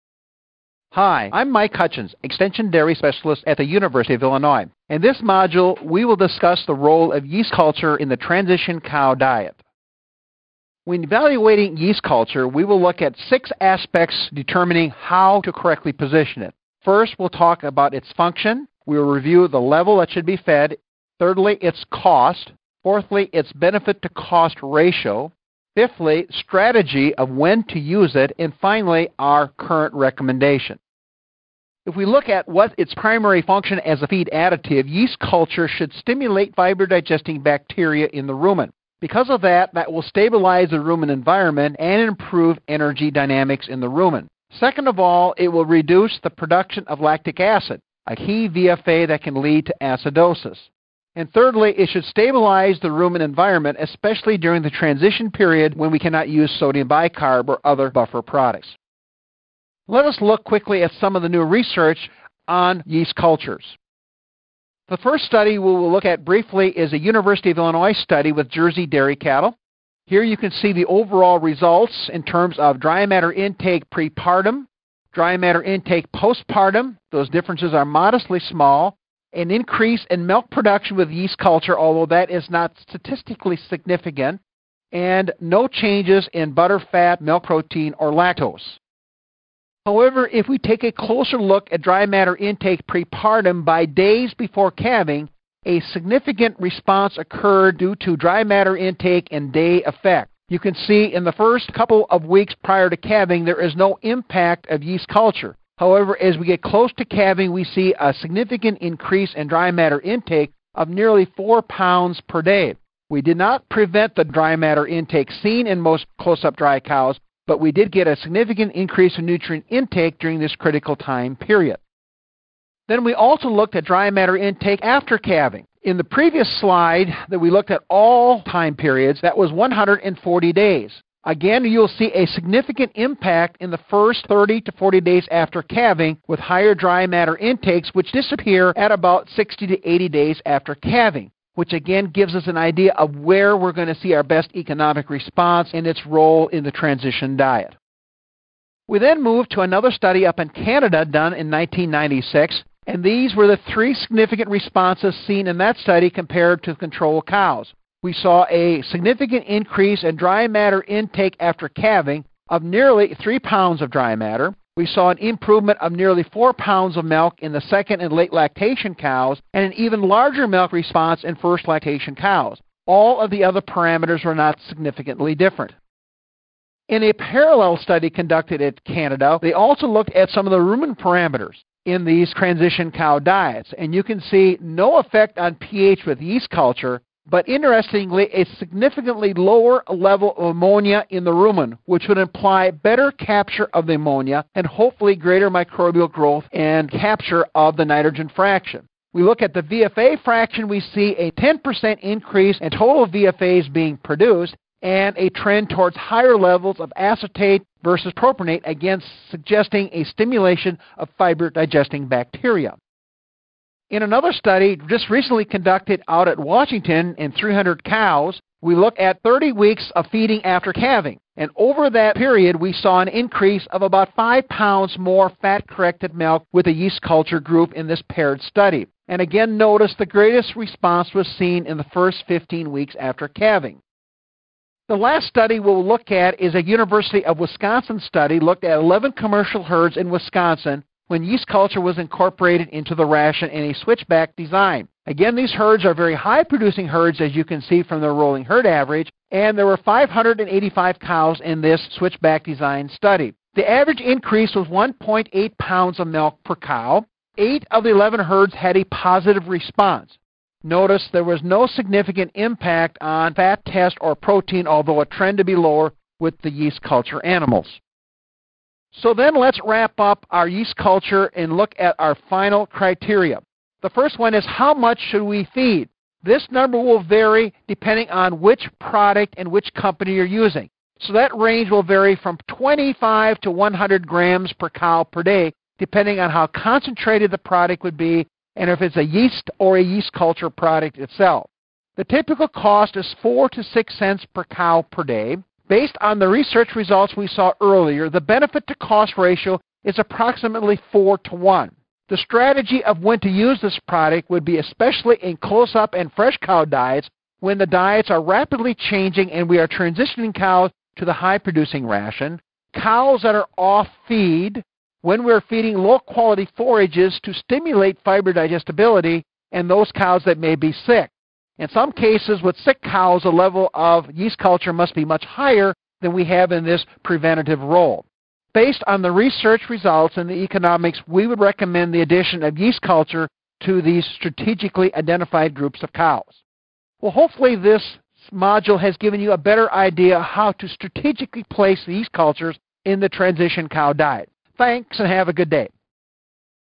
Role of Yeast Culture in Transition Cow Diets
Mike Hutjens, Extension Dairy Specialist at the University of Illinois, discusses role of yeast culture in transition cow diets.